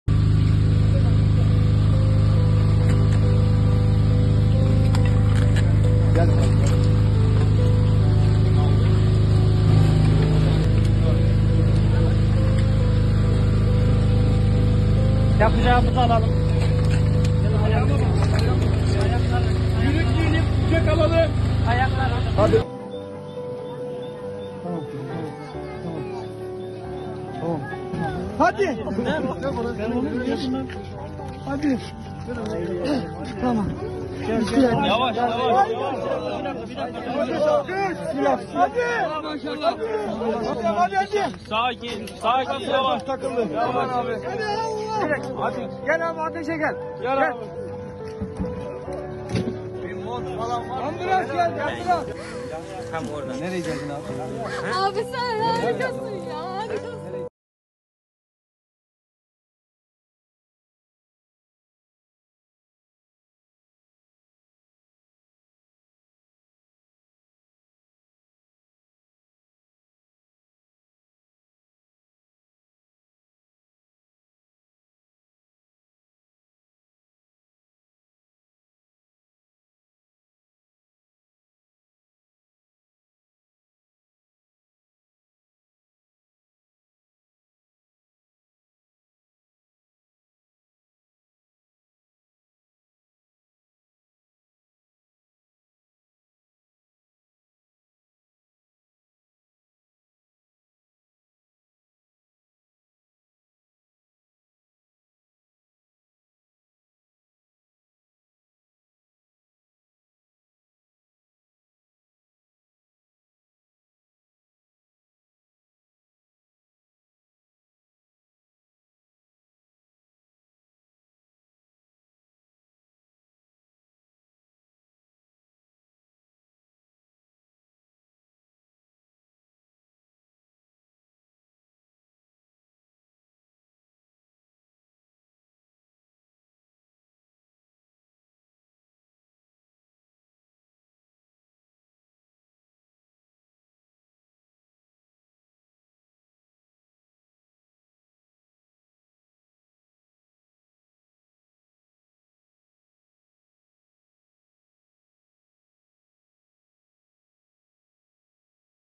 Özel Harekat Umutlarımızı Yeşertti! Depremzede Polise Böyle Teşekkür Etti - TGRT Haber
Ülkeyi yasa boğan deprem felaketinin ardından tüm Türkiye seferber olurken, İstanbul İl Emniyet Müdürlüğü’ne bağlı polisler Kahramanmaraş merkezli depremden etkilenen illerde arama kurtarma ve enkaz kaldırma çalışmalarına katıldı. Hatay’da deprem nedeniyle çöken bir binada mahsur kalan 3 yaşındaki bir kız çocuğu ile genç bir vatandaş özel harekat timlerinin çalışmasıyla sağ olarak kurtarılırken, polisler, enkazdan çıkardıkları çocuğu kucağında taşıyarak ambulansa götürdü.